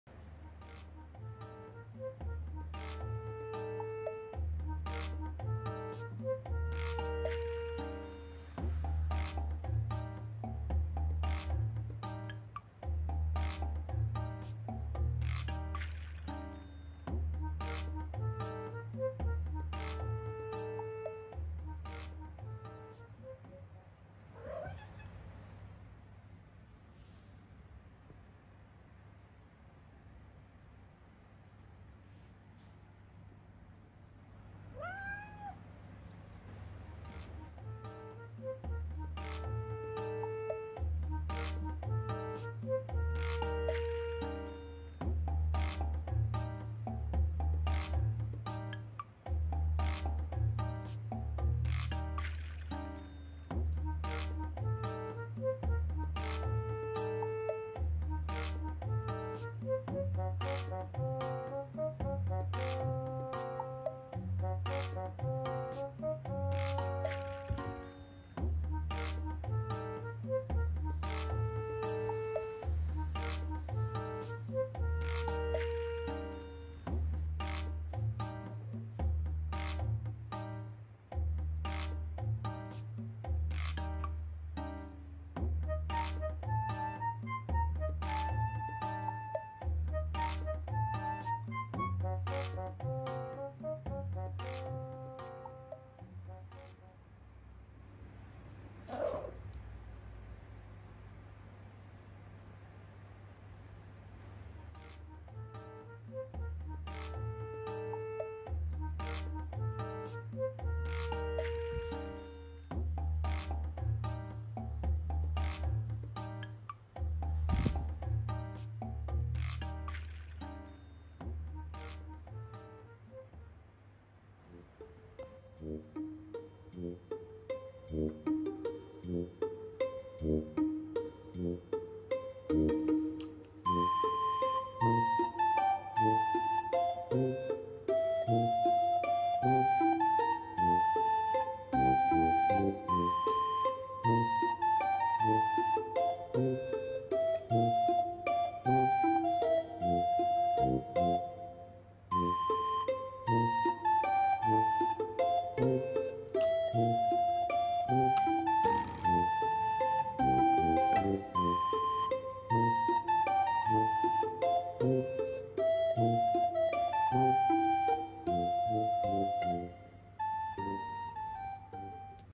ペットボトルアタックにゃんず
保護猫
三毛猫　きなこ　♀
茶トラ　にゃん太　♂
との何気ない日常